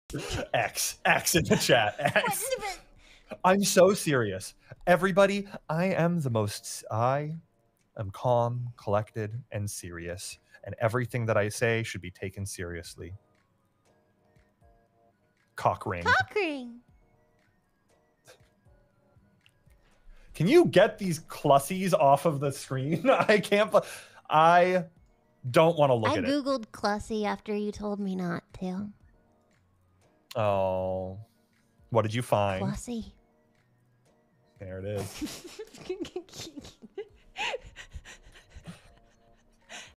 Lewdcast feat Drumsy- Cockrings and Clussies
Contrary to what he claims, very few things Drumsy says should be taken seriously. And get these damn clussies out of here.

for more random nonsense.